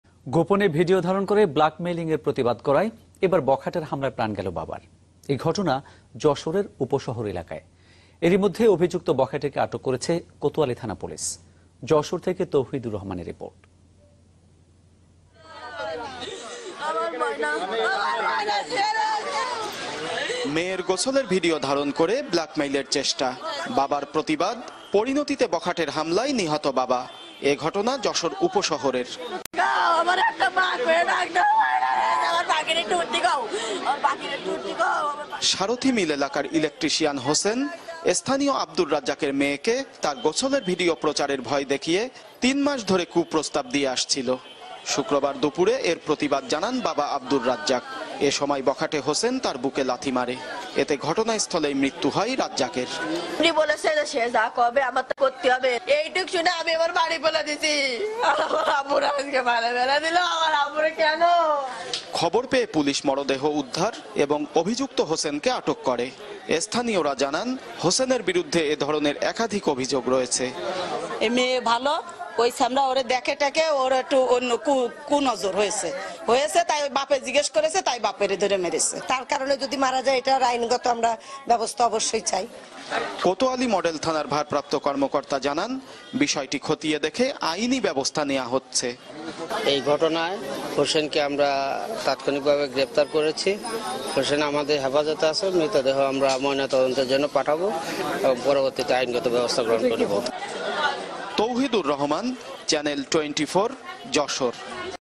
যশোরে মেয়ের গোপনে ভিডিও করে ব্লাকমেইল করে বখাটেরা, প্রতিবাদ করায় প্রান গেল বাবার | Jessore Uposohor
Copyright - Channel 24
যশোরে মেয়ের গোপনে ভিডিও করে ব্লাকমেইল করে বখাটেরা, প্রতিবাদ করায় প্রান গেল বাবার | Jessore Uposohor Update News |